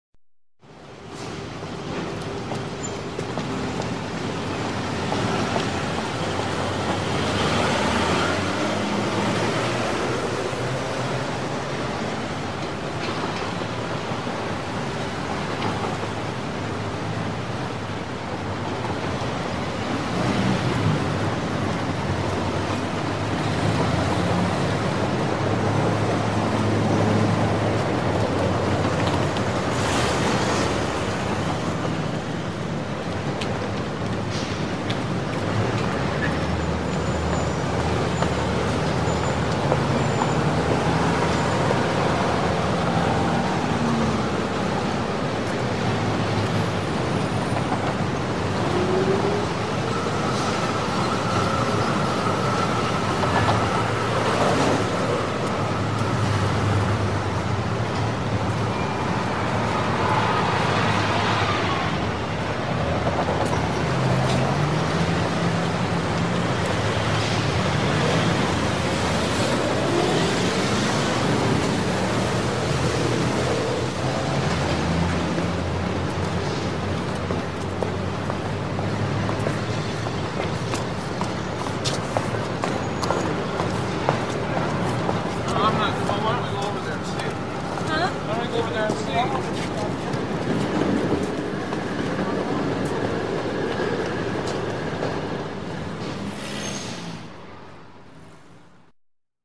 City Hubbub | Heavy Urban Traffic Ambience | Free Editing Sound Effect
Step into the vibrant chaos of urban life with our latest sound effect clip! Immerse yourself in the dynamic soundscape of heavy city traffic, where cars, buses, voices, and footsteps create a lively and bustling atmosphere. This audio clip is a fantastic addition to cityscape montages, street scenes, and more. At [Your Channel Name], we're committed to providing you with premium sound resources that enhance your creative projects.